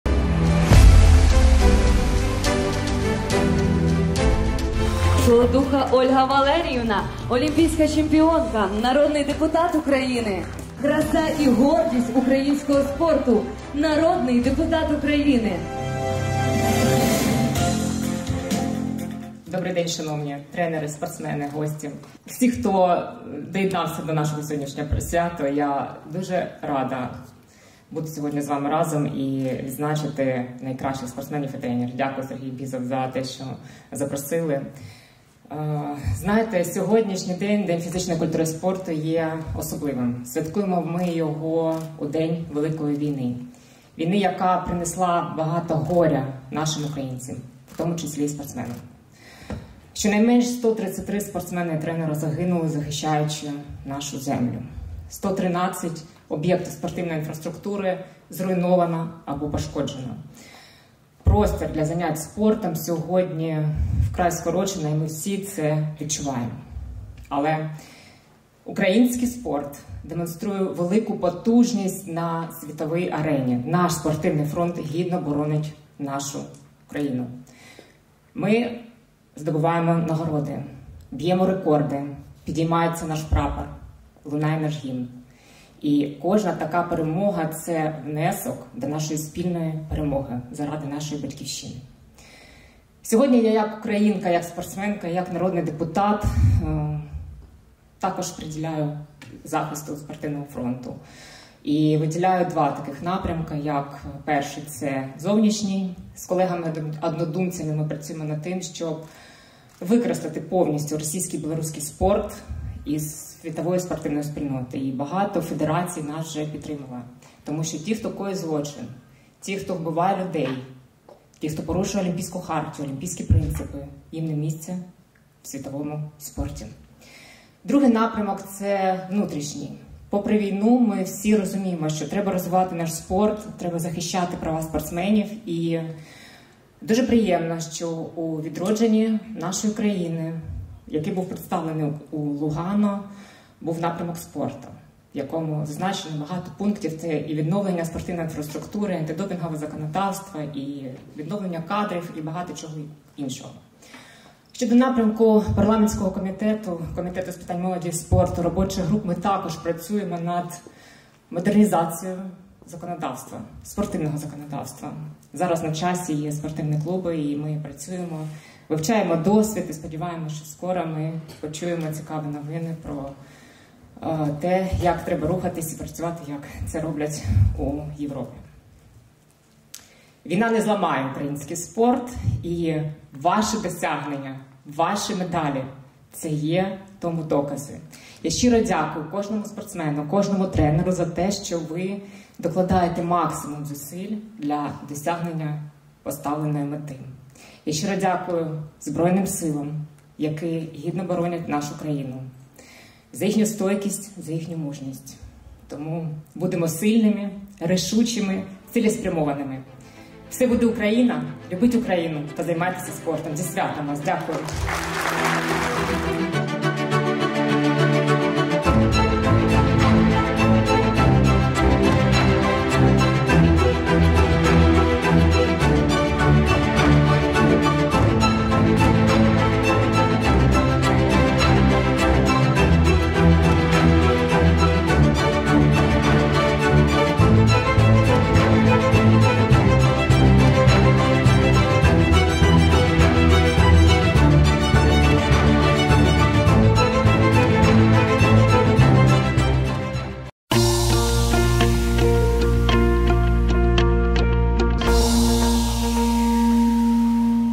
російсько-білоруський спорт ми викинемо зі світової спільноти, - О.Саладуха
За час повномасштабної війни росія знищила  в Україні близько 100 спортивних об'єктів, ще з десяток лишаються пошкодженими. Тому дуже важливим є виключення росії та білорусі з усіх можливих світових споривних майданчиків за боряться народні депутати у правовому полі.